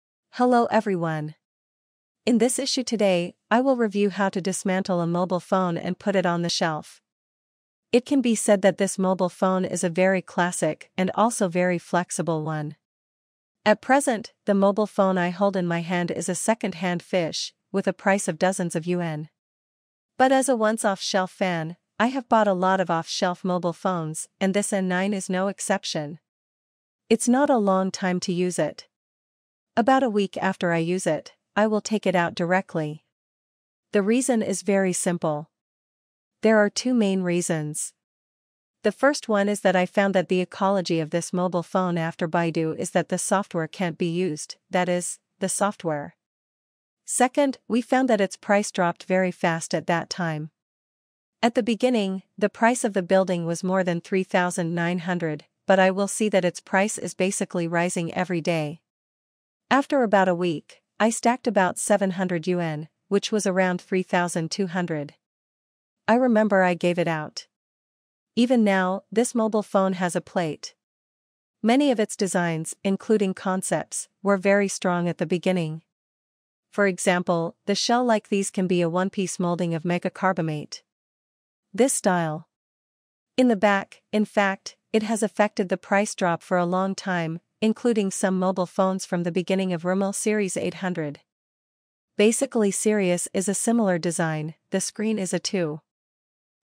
Nokia N9 -Unboxing and review - Bought on amazon
This is an unboxing and setup video for I purchased on Amazon.  More information about this Product can be found at the following link.